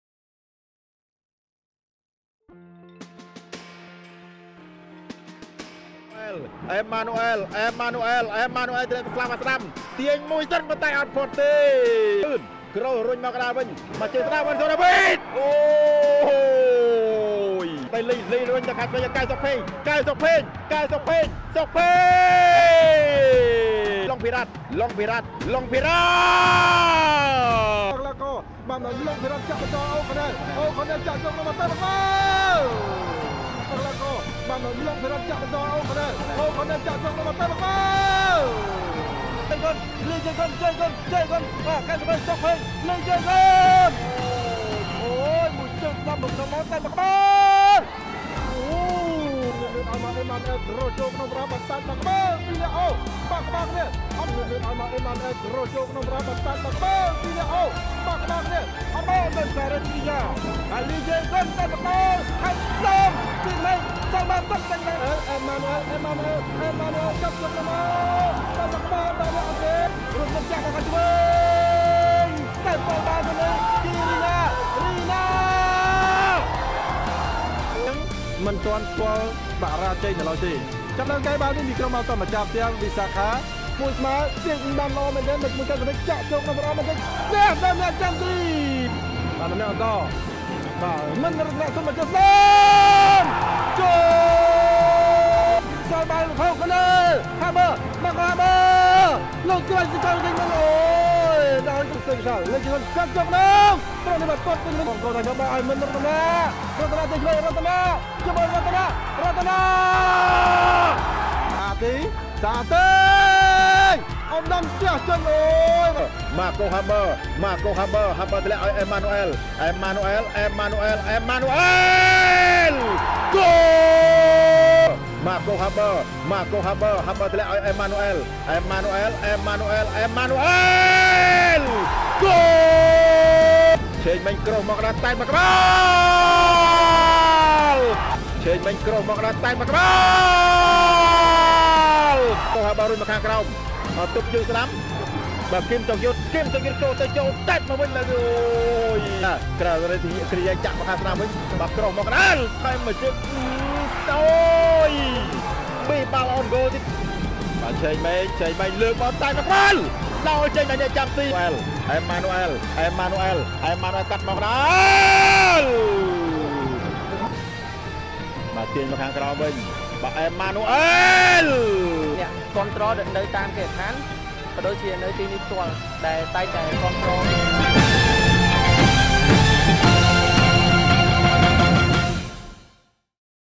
Highlight: Visakha FC (1-2) Preah Khan Reach Svay Rieng FC | CPL-WEEK7
April 23, 2022  Cambodian Premier League 2022 (Week7)
Video highlight:
Visakha FC (1-2) Preah Khan Reach Svay Rieng FC
Prince Stadium | Kickoff 6:30pm
Broadcast by: Troyorng